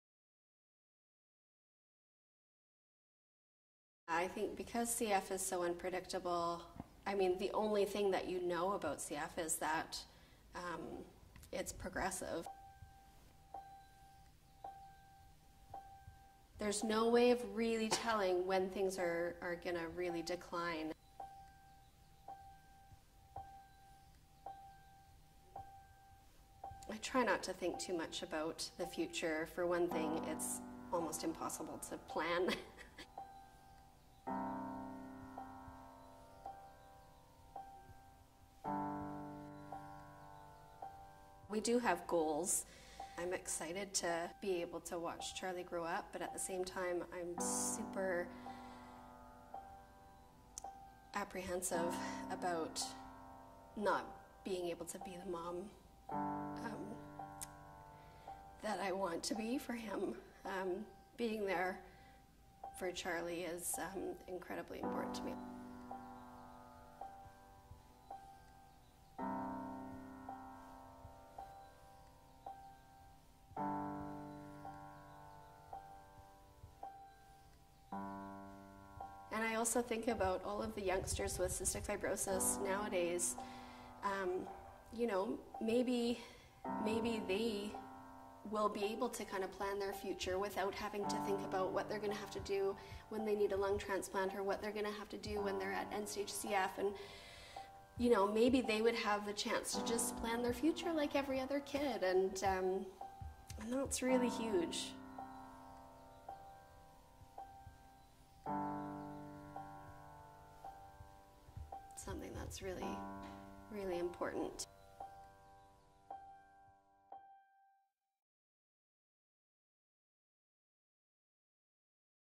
Kim Wood: Whatever it takes; to be a mom for as long as I can
Now is the time to tell candidates to take a stand and develop a strategy facilitating access to life-changing medications for people with rare diseases, including cystic fibrosis. Help us do whatever it takes for Kim and her family, sign and share the petition now